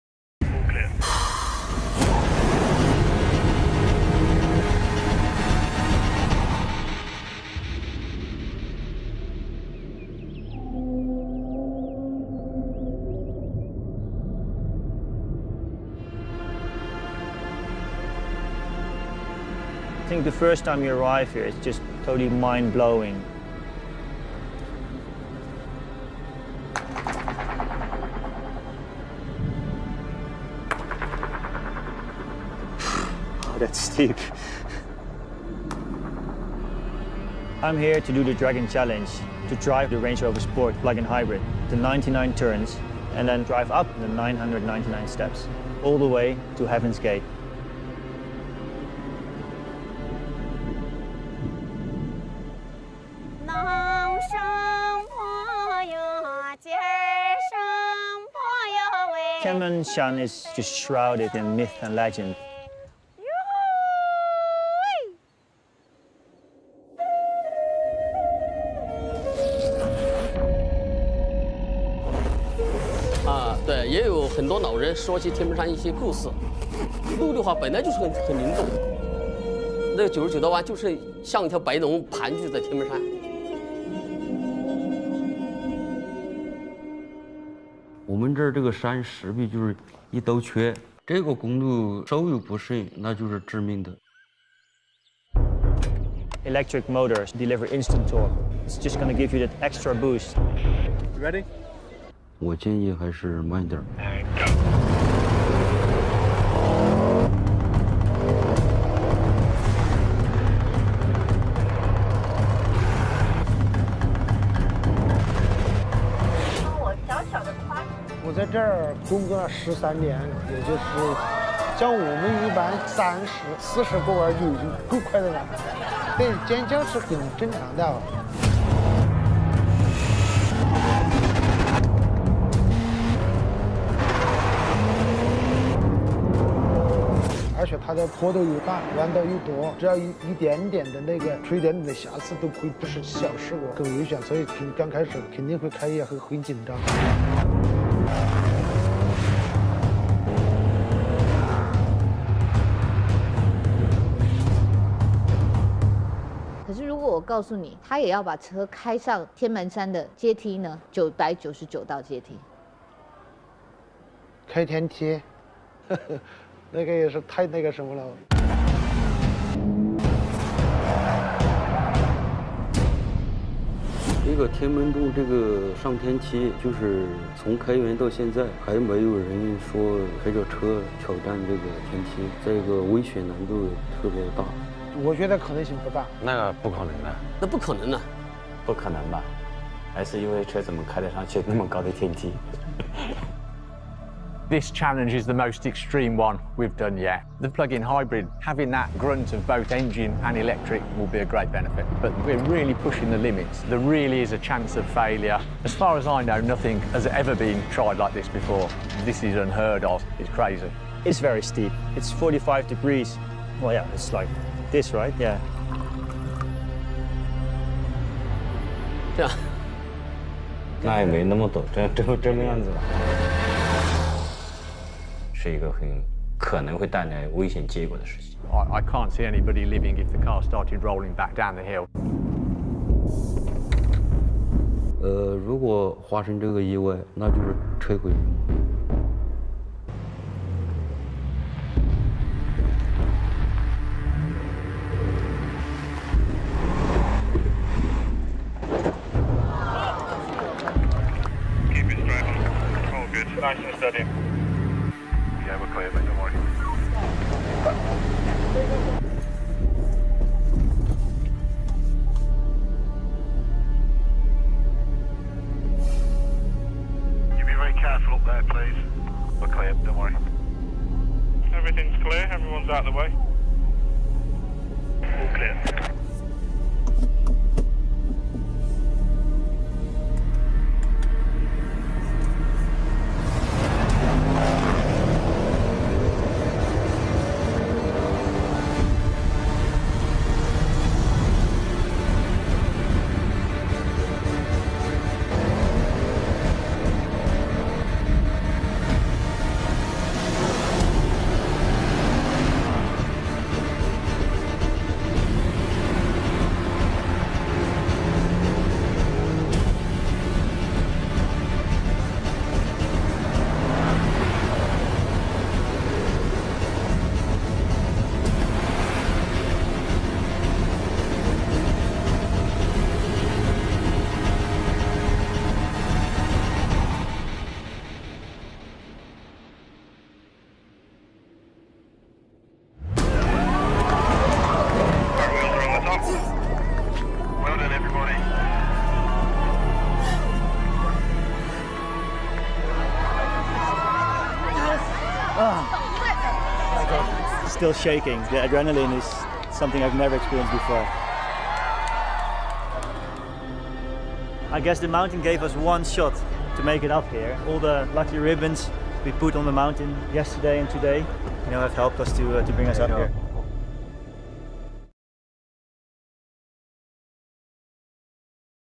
Courage is Everything that is Needed | Happy New Year
Range rover stair challenge, it is one of the best suv in the world. Best courage video. Car adventure.